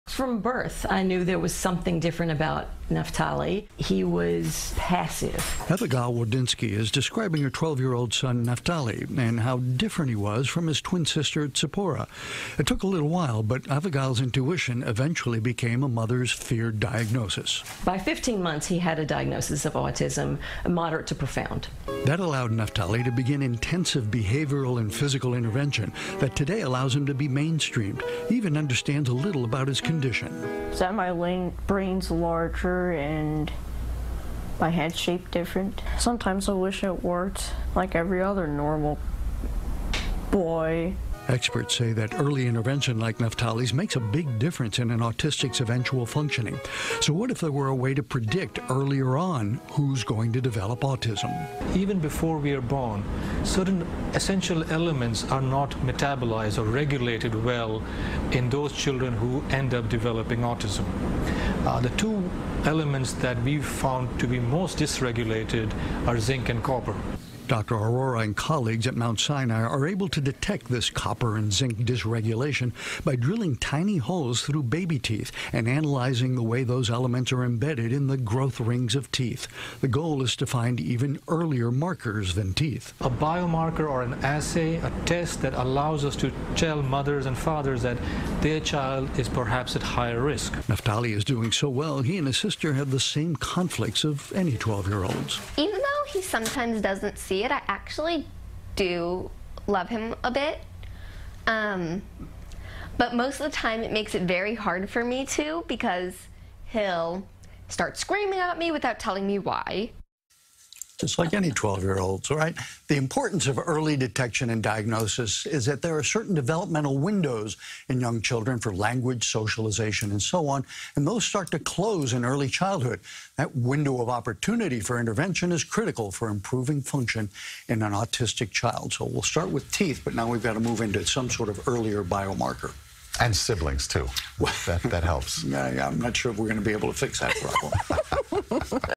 Baby Teeth New Option For Predicting Autism
Baby teeth start to form around the end of the first trimester of pregenancy, and the signatures present can predict the emergence of autism later in childhood. CBS2's Dr. Max Gomez reports.